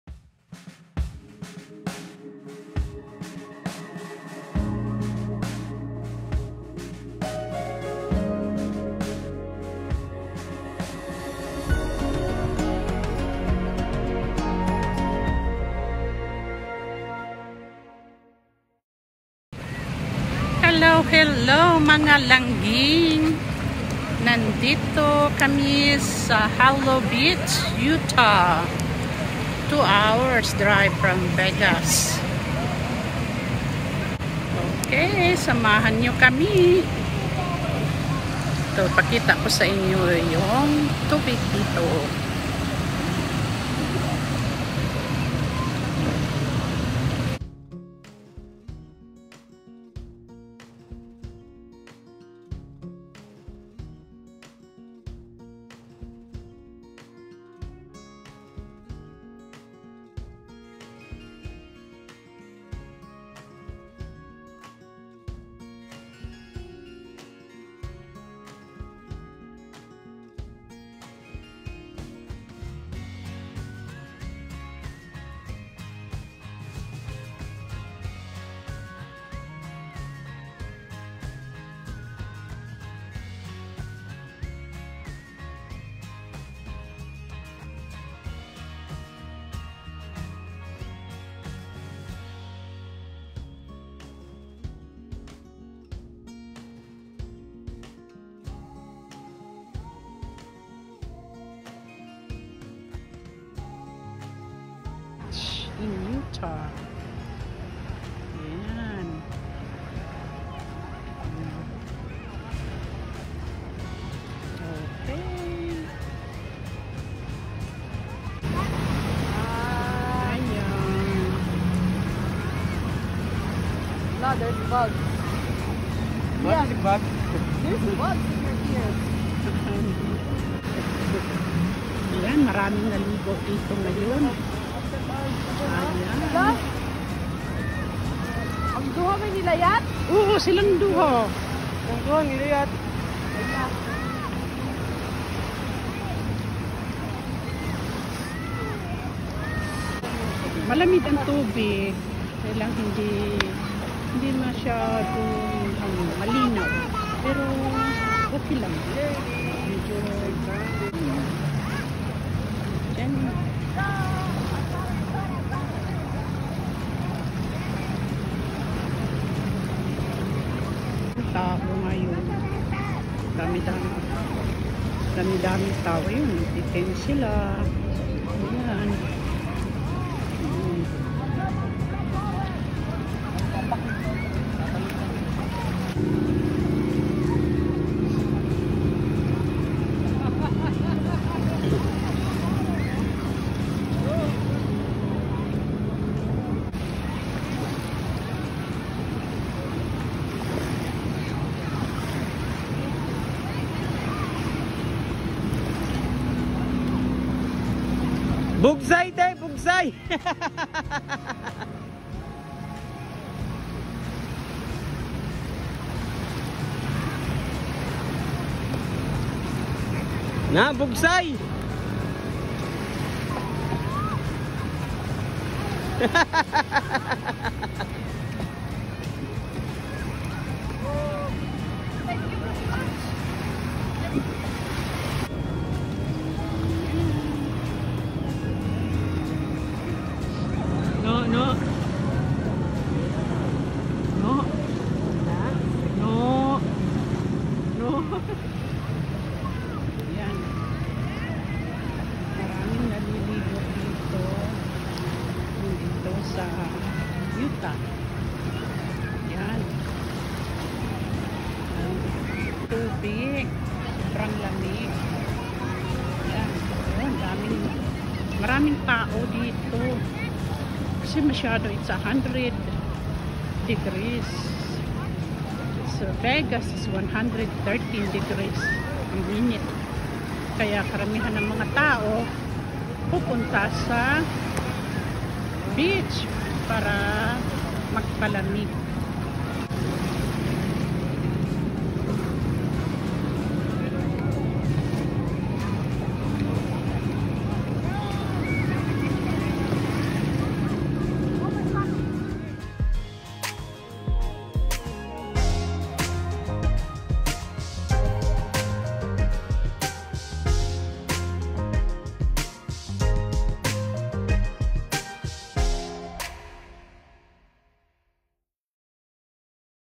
Vlog #9 Lets Go Kayaking and Swimming at Sand Hollow Beach State Park, Utah
We visited Sand Hallow Beach, Utah. Beautiful weather with an amazing beach! Utah is four Hours drive in Las Vegas Nevada. Family and friends bonding. Great kayaking and swimming. Let’s go kayaking and swimming in Sand Hollow Beach Utah.